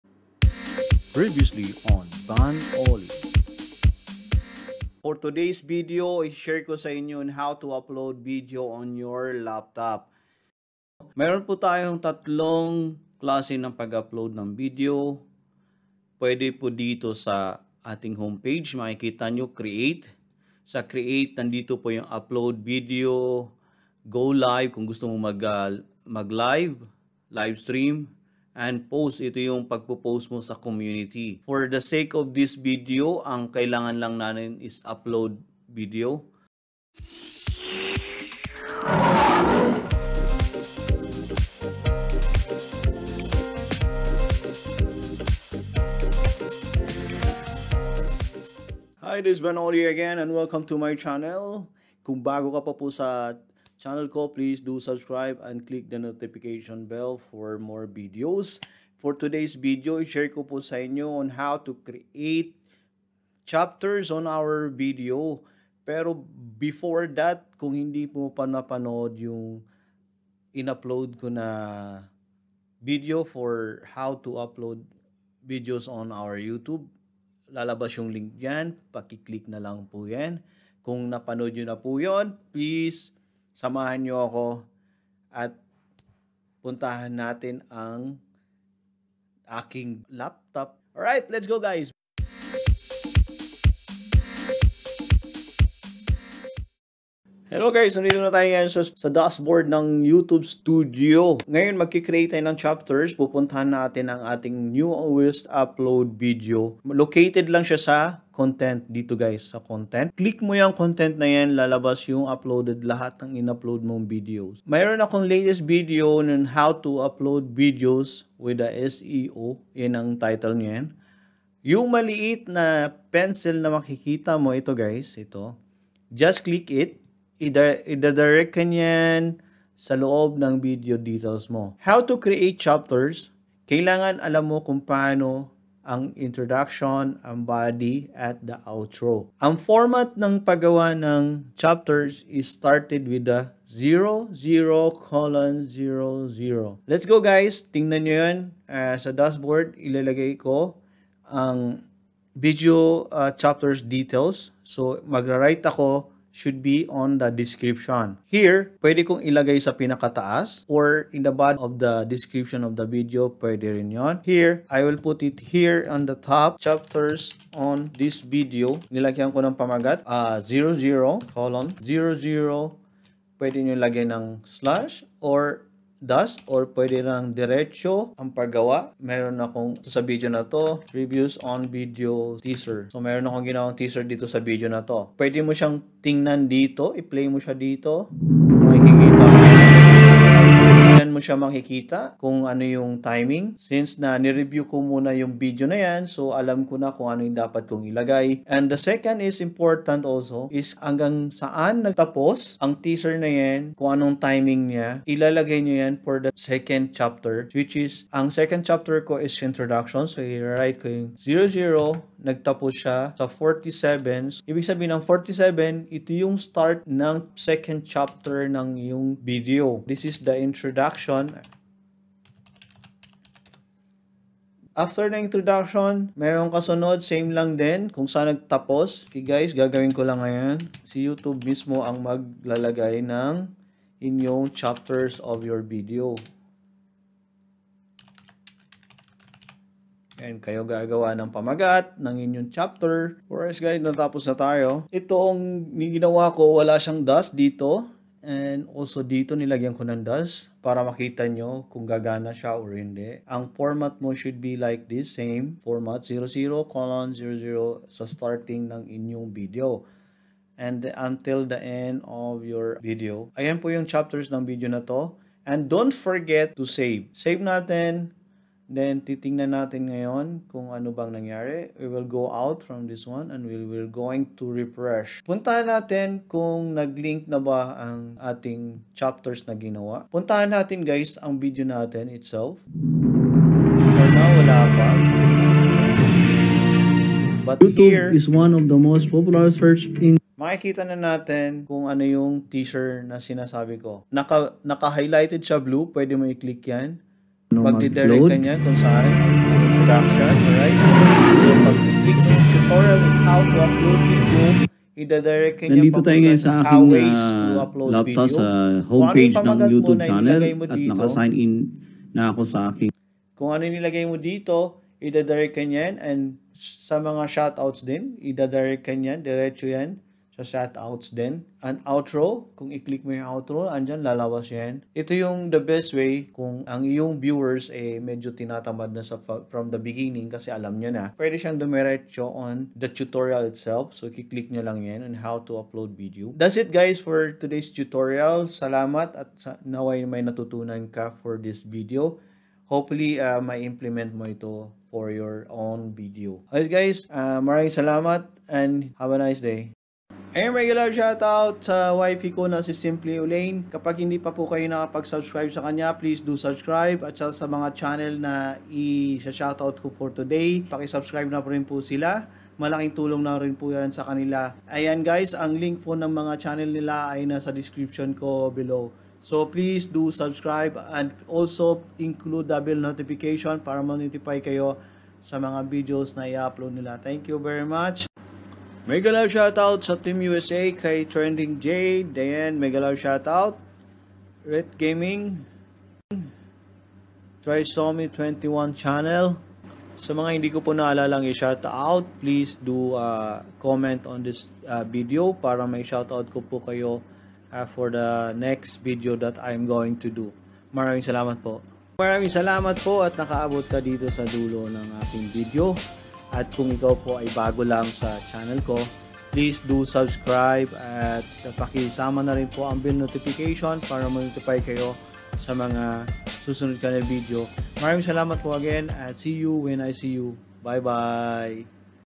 How to Add Chapters in YouTube Video for Beginners Guide || Step by Step Tutorials || Van Oli
How to Add Chapters in YouTube Video for Beginners Guide || Step by Step Tutorials || @VanOli25

In this video provides tutorial on how to add chapters in youtube videos for beginners guide step by step .

What is the format of YouTube Chapters?
► 00:00 - Text
► 00:XX - Text

What is Chapter on YouTube Videos?
► YouTube Video Chapters are generated through on the timestamps that creators include in description of your video's.

Chapters on this video:
00:00 - Previous Video Teaser
00:35 - Introduction
01:32 - How to Create Chapters on Video
05:50 - Shout-outs
06:42 - Outro